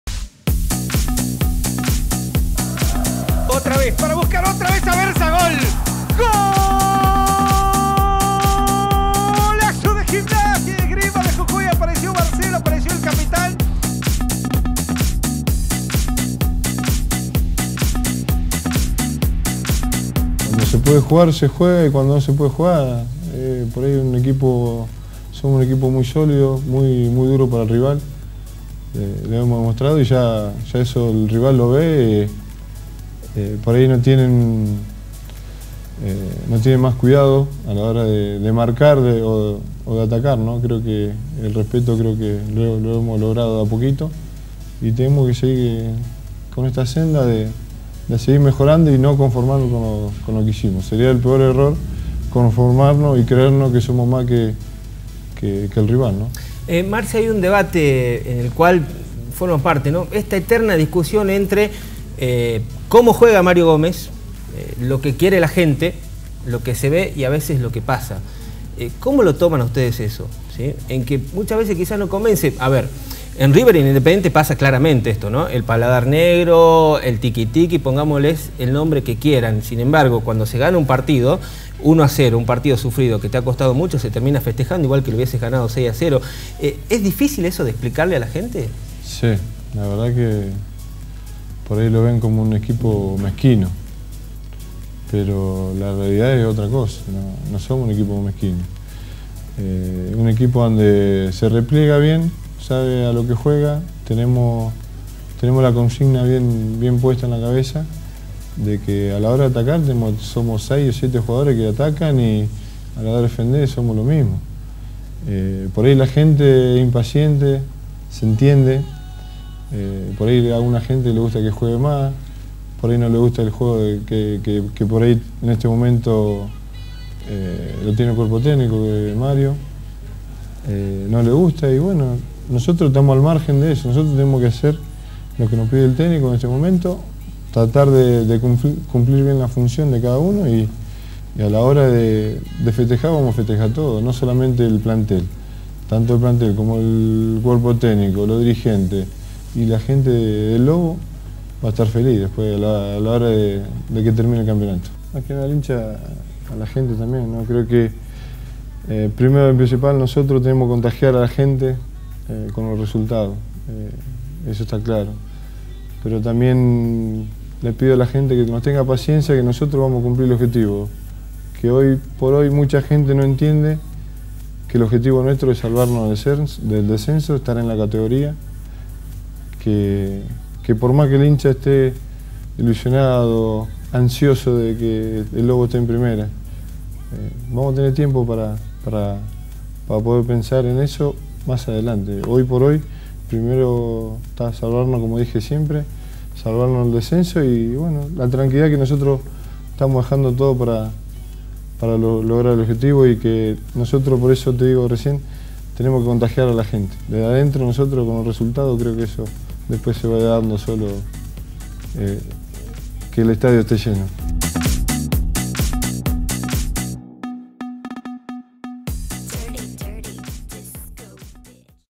Marcelo Berza vuelve al equipo titular de Gimnasia de Jujuy
El capitán y defensor del "Lobo jujeño" habló en Canal 4 deportes de el estilo futbolístico del equipo y pidió por el apoyo de los hinchas locales.